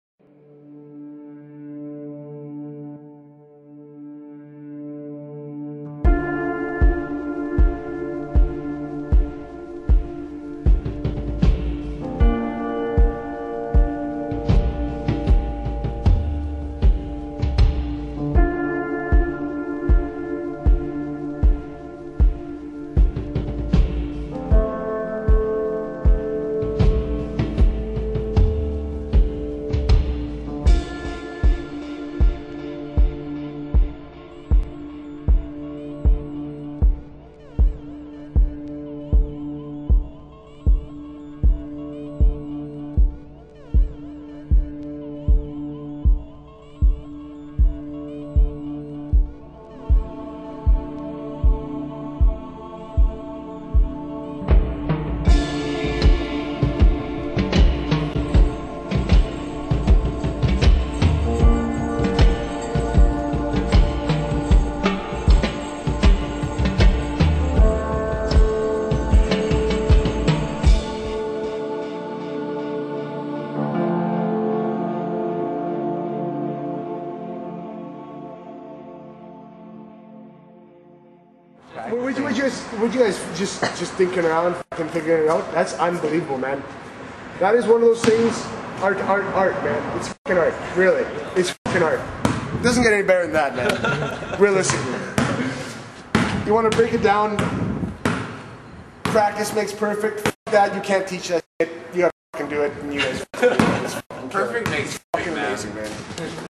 Poetry in Motion Intro - My Freestyle Journey | Snake
The intro for Poetry in Motion showing bits of what's gone into making Snake the freestyler he is now.